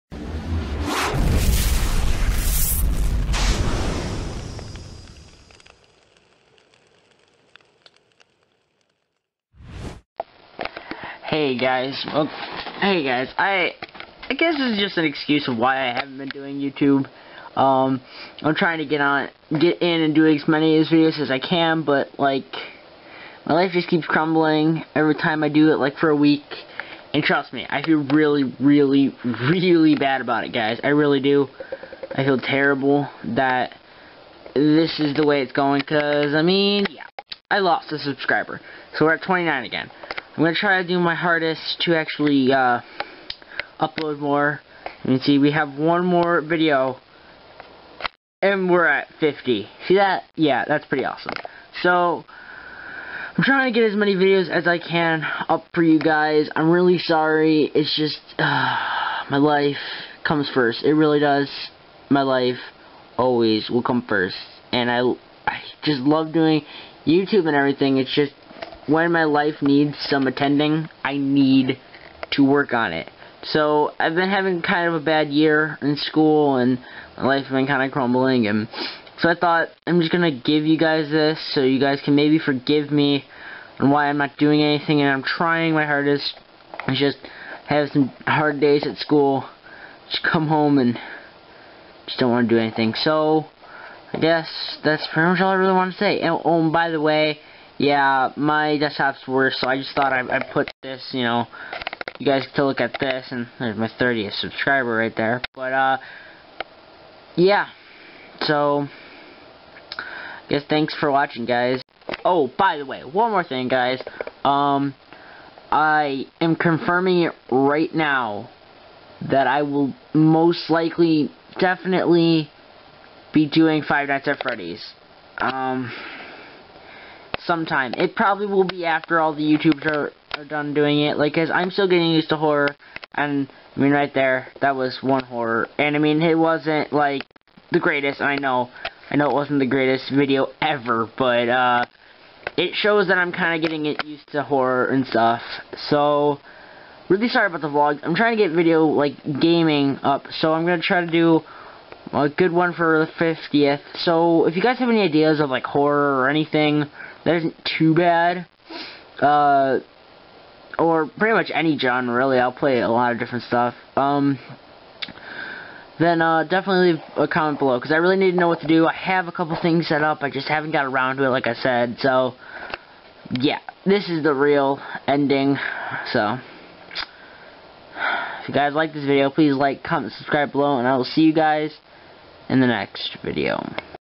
Apologies about everything
my Channel is suffering and it sucks
so im sorry for no videos
my life is really in a bad place
and im hoping to get vids up soon
sorry i have been doing Vlogs lately as well i just have to comunicate some how :P